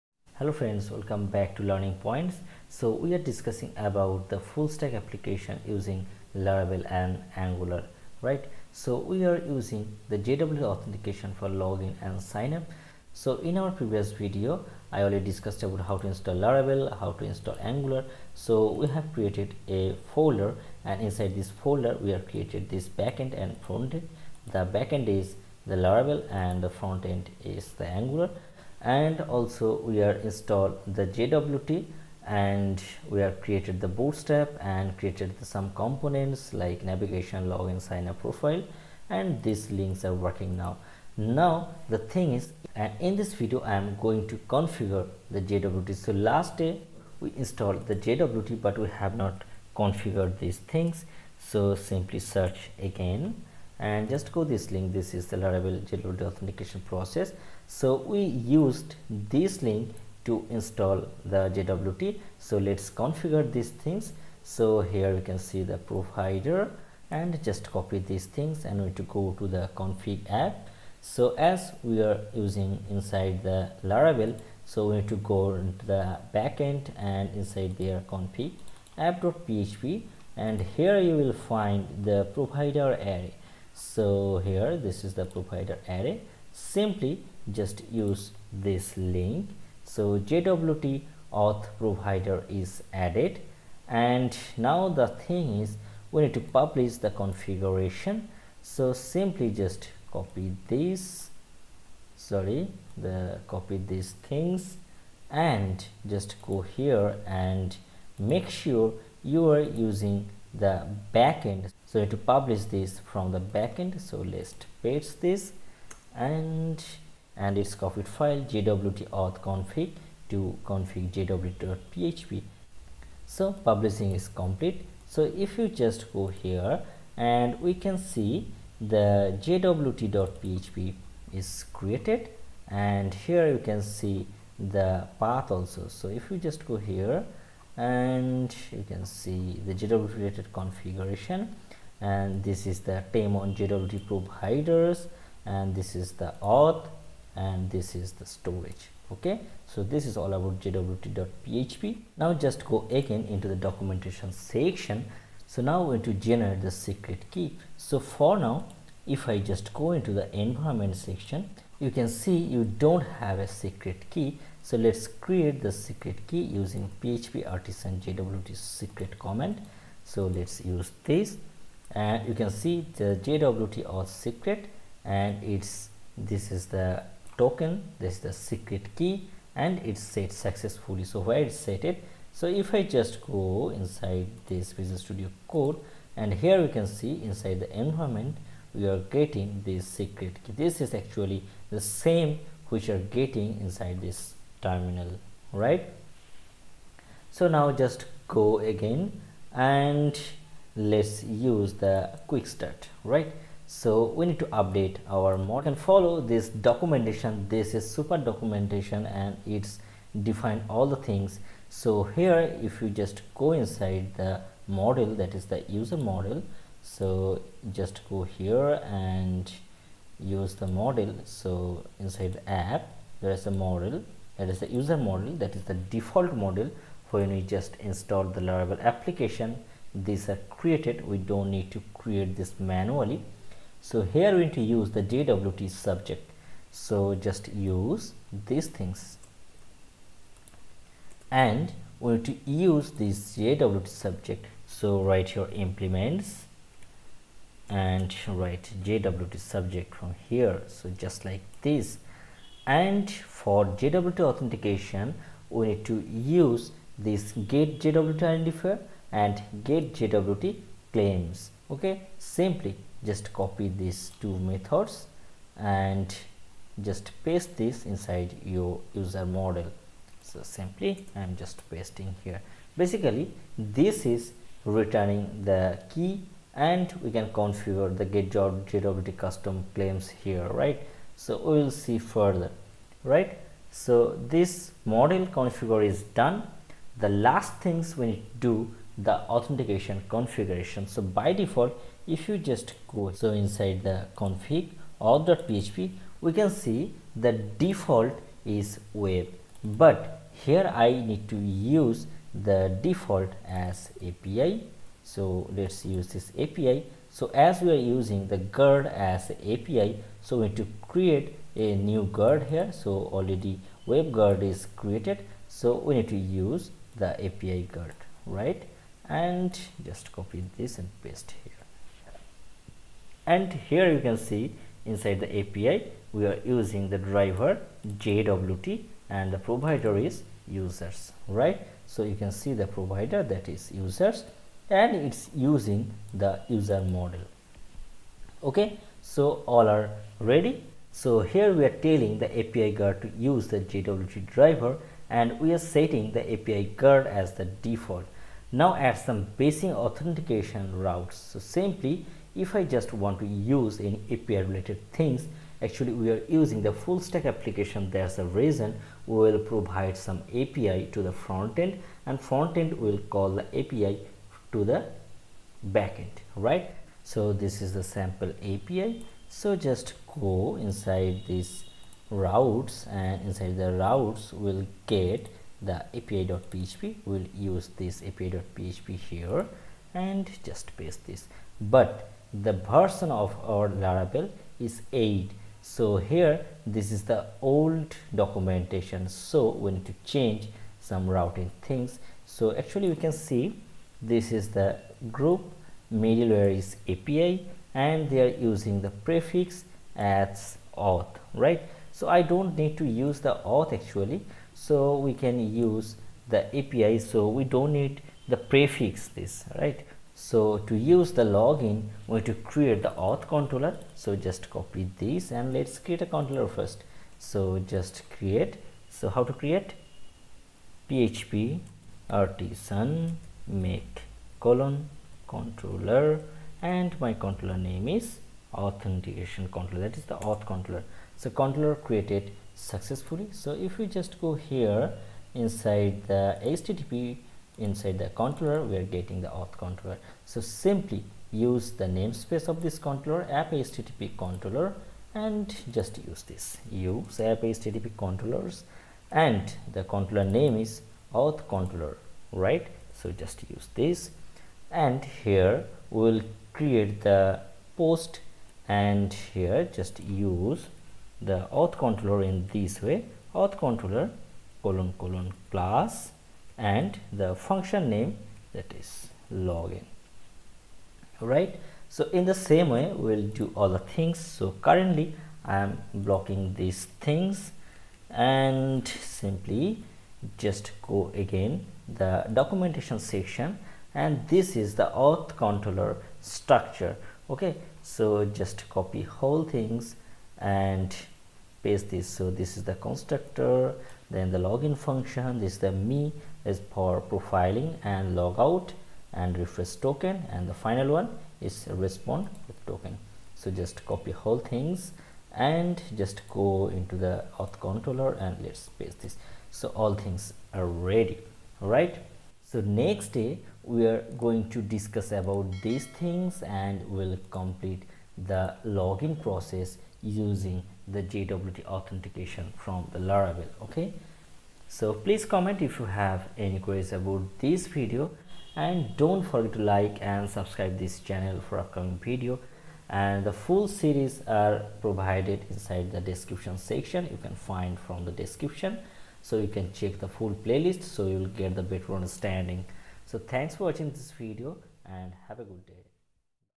Full Stack Application with Laravel and Angular | JWT setup and Configure of model, route and auth
JWT setup in laravel application  Configuration of model, route and authenticatication | Full Stack Application with Laravel and Angular | Learning Points

In this video we will learn how to setup JWT into laravel. After that configure route, user model and authentication.
Next day we are going to Create Login Form